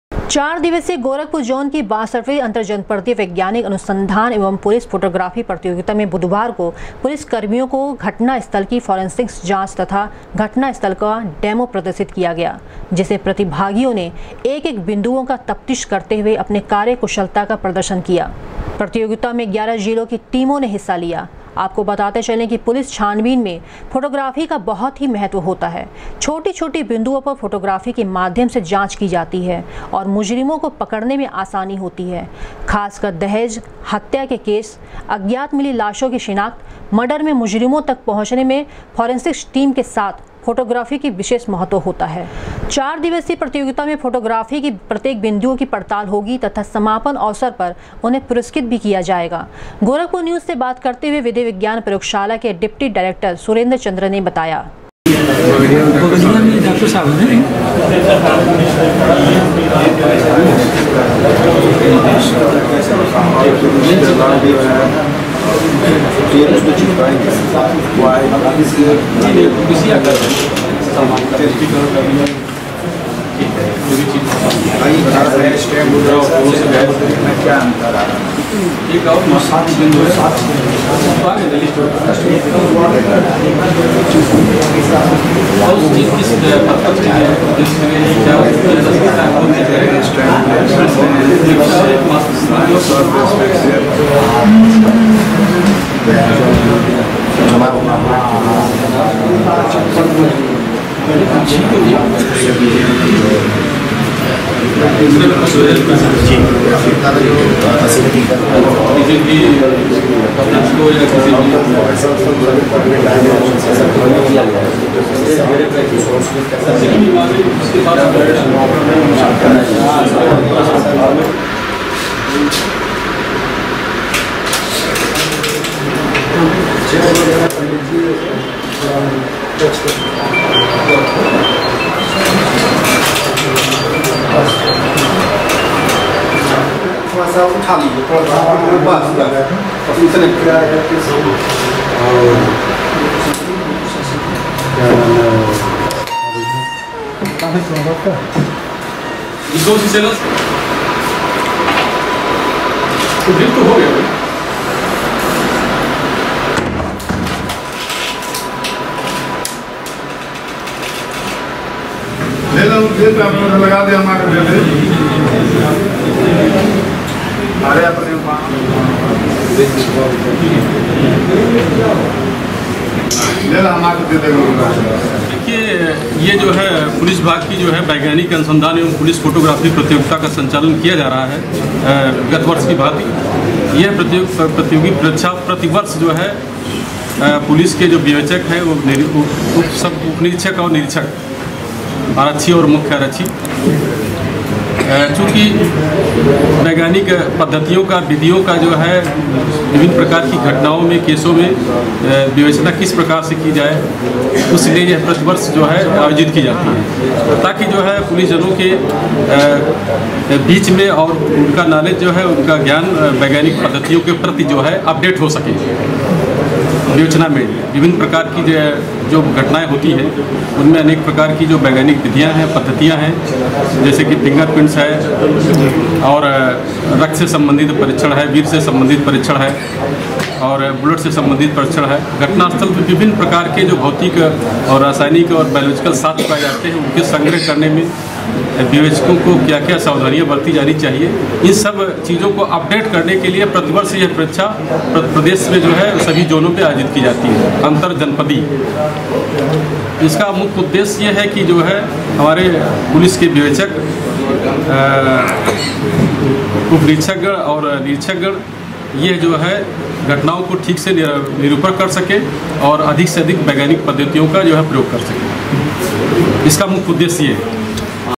अंतर्जनपदीय वैज्ञानिक अनुसंधान एवं पुलिस फोटोग्राफी प्रतियोगिता के तीसरे दिन मर्डर केस् के
अंतर्जनपदीय वैज्ञानिक अनुसंधान एवं पुलिस फोटोग्राफी प्रतियोगिता  के तीसरे दिन मर्डर केस् के जाँच की हुई डेमो, डिप्टी डायरेक्टर ने कहा पुलिस की जाँच में फोटोग्राफी का विशेष है महत्व